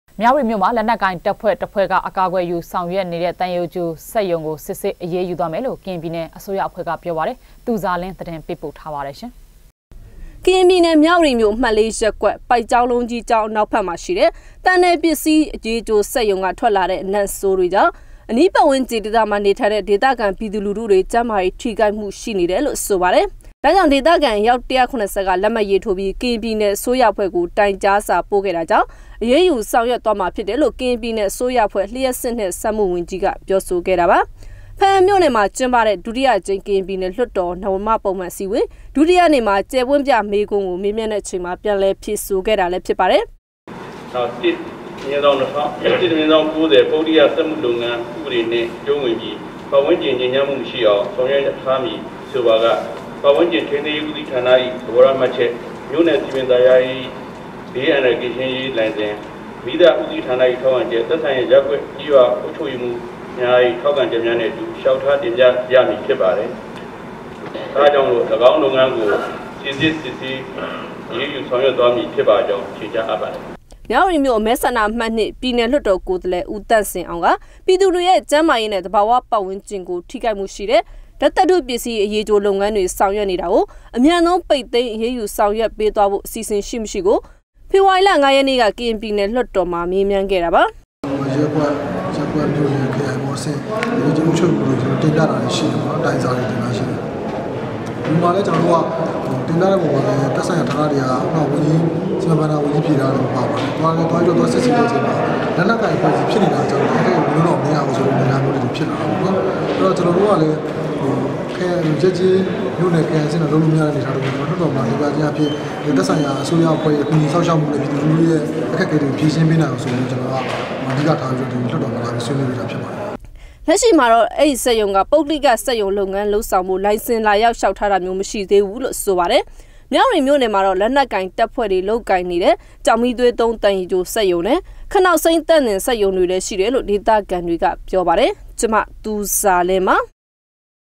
DVB - လက္နက္ကိုင္ တပ္ဖြဲ႔ အကာကြယ္ယူထားတဲ့ သံရည္ၾကဳိစက္ရုံကို စစ္ေဆးအေရးယူမယ္
ျမ၀တီၿမိဳ႕မွာ လက္နက္ကိုင္တပ္ဖဲြ႔ တစ္ဖဲြ႕က အကာအကြယ္ယူေဆာင္ရြက္ေနတဲ့ သံရည္ႀကိဳစက္ရံုကို စစ္ေဆးအေရးယူသြားမယ္လို႕ ကရင္ျပည္နယ္အစိုးရအဖဲြ႕က ေျပာပါတယ္။ သူဇာလင္းေပးပို႕ထားပါတယ္။